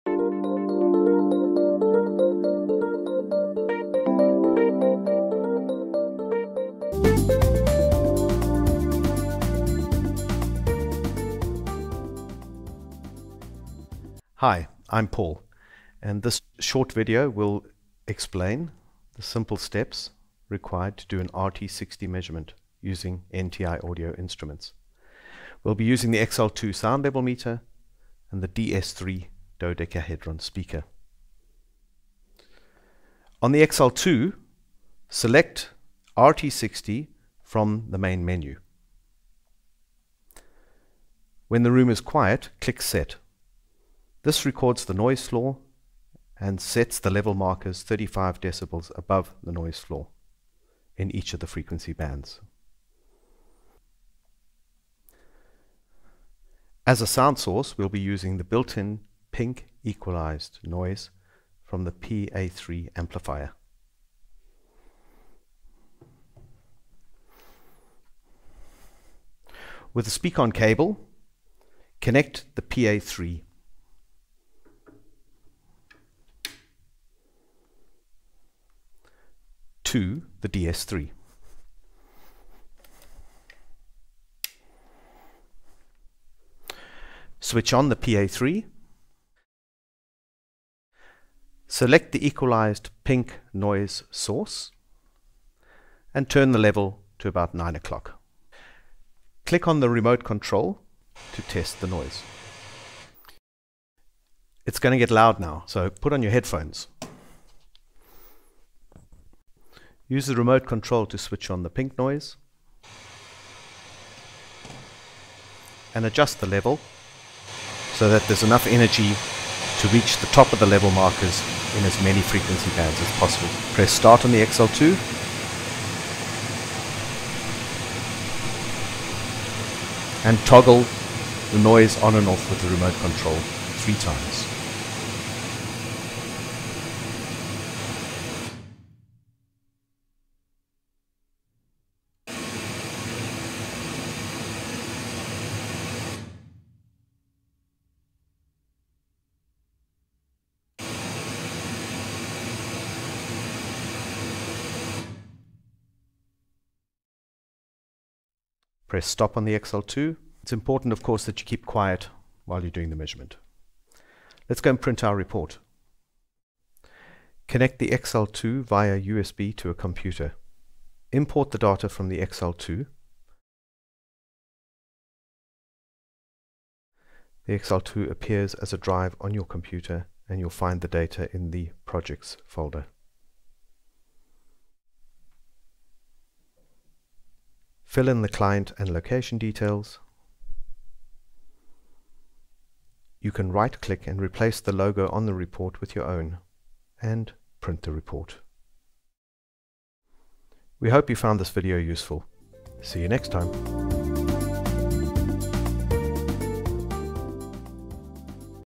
How to measure Reverberation Time RT60 (old version)
How the report is generated (Excel file) in this video is no longer accurate. Please watch the new version of the video

This video explains how to measure reverberation time RT60 with the XL2 Audio Analyzer, DS3 Dodecahedron Speaker and the PA3 Power Amplifier.
Der Vorgang, wie ein Report erstellt werden kann, ist nicht mehr aktuell. Bitte sehen Sie sich dafür die neue Version des Videos an

In diesem Video zeigen wir, wie die Nachhallzeit RT60 mit dem Audio Analysator XL2, Dodekaeder-Lautsprecher DS3 und dem Leistungsverstärker PA3 gemessen wird.